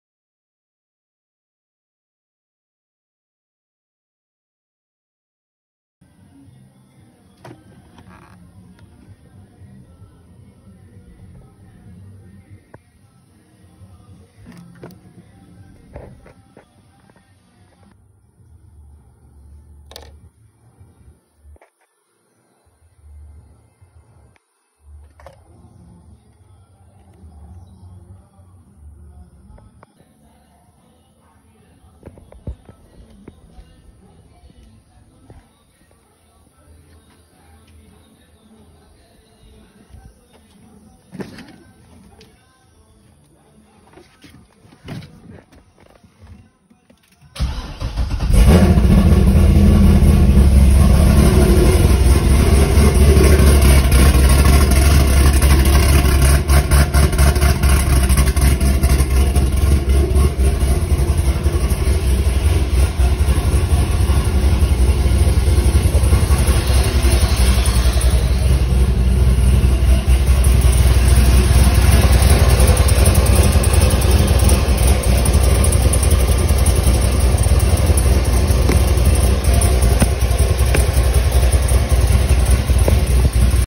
C6 corvette fuel pump relay update.
Failed to start again turned out it was making a bad connection tore it apart bent it back and back to normal.